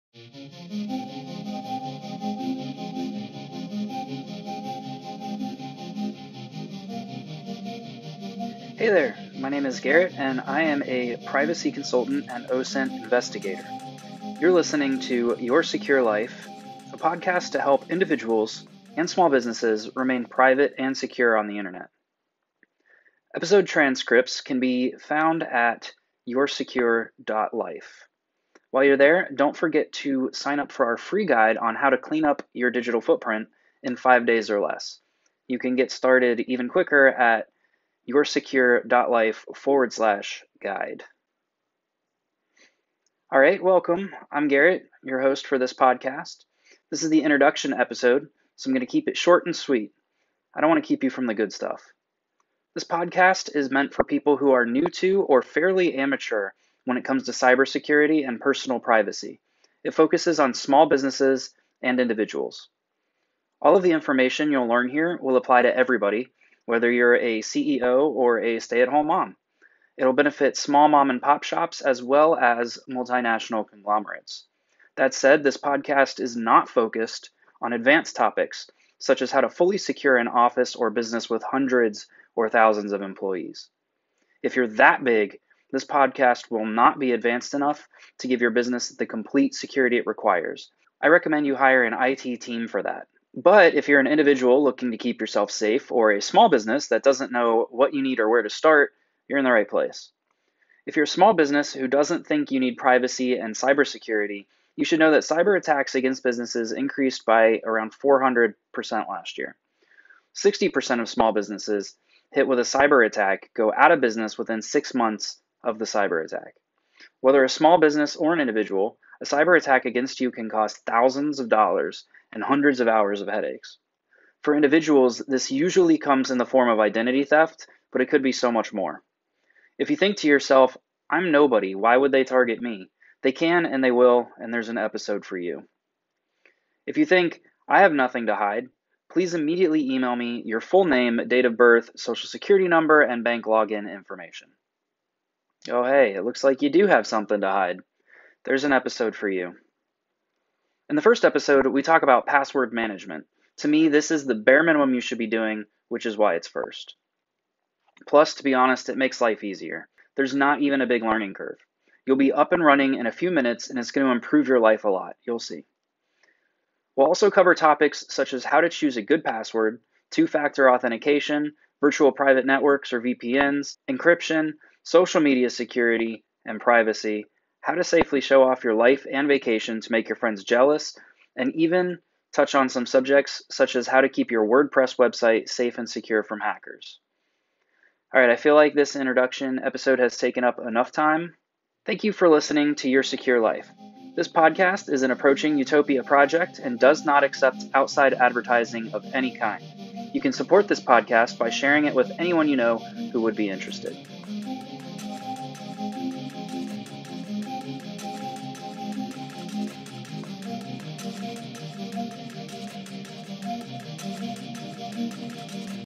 0. Introduction to Your Secure Life Podcast
Garrett introduces the Your Secure Life podcast.